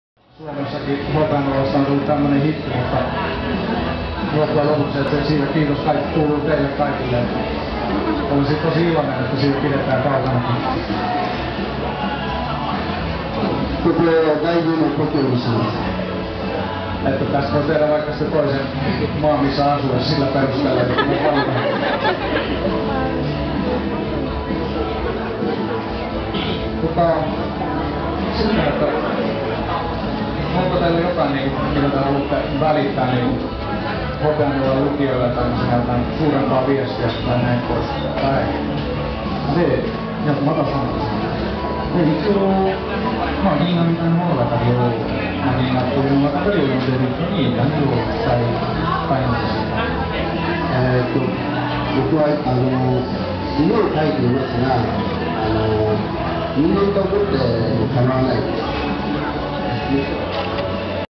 Yoshihiro Takahashi Helsingissä syyskuussa 2011
Ihan lyhkänen klippi Yoshihiro Takahashin haastattelusta Kampin Suomalaisessa kirjakaupassa.

Very short clip of Yoshihiro Takahashi being interviewed in a bookstore in Finland.